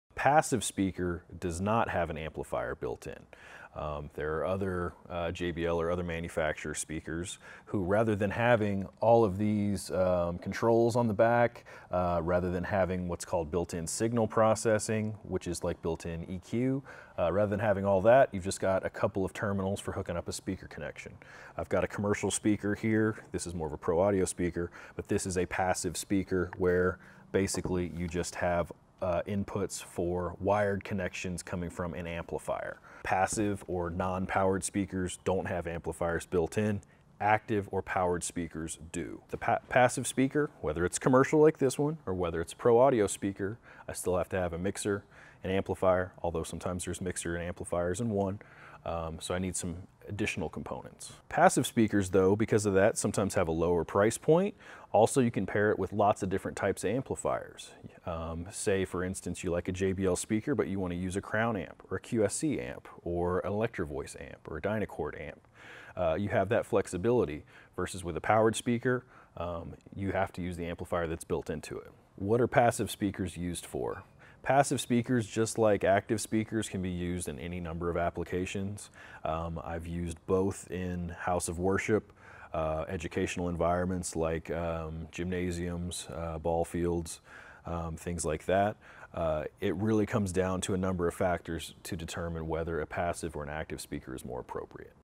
Quick Clip: What are passive PA speakers? (Pro Acoustics Tech Talk Ep. 55)
Passive PA speakers do not have built-in amplifiers that require them to be plugged into AC electricity. However, this also means that you'll need an external audio amplifier to provide them with power.

What Are Passive Speakers (Also referred to as “unpowered”)
- Passive speakers rely on external amplifiers
- Passive speakers do not require AC power to function
- More versatile in terms of amplifier matching
- Requires more gear and cabling- they need speaker wire run from the amplifier. They will also need a mixer where you will connect your music source or microphone before the amp.

Pros
- Can be paired with your choice of different types or brands of amplifiers
- Fewer wires without the power cord making them easier to place
- Relatively easy to upgrade inside your system since you can switch out  the amplifier, mixer, or processing as needed
- They’re usually lighter weight
- Lower in price
- Easier maintenance

Cons
- The user is responsible for amplifier matching
- Needs a mixer or additional processing like EQ
- A lot more components involved in your sound system
- Often uses unbalanced audio

What are passive speakers used for?
Passive speakers, just like active speakers, can be used in any number of applications. Both can be used in houses of worship, educational environments, gymnasiums, ballfields, etc. It comes down to several factors to determine whether a passive or an active speaker is appropriate.

Consider unpowered/passive speakers if…
- You might want to upgrade your amplifier at a later date
- You’ve already invested heavily in the rest of your system components
- You’re more restricted in your budget
- Weight is an issue

If you like this video, give us a thumbs up and subscribe to our channel to be the first to know when we post new content about audio products and design.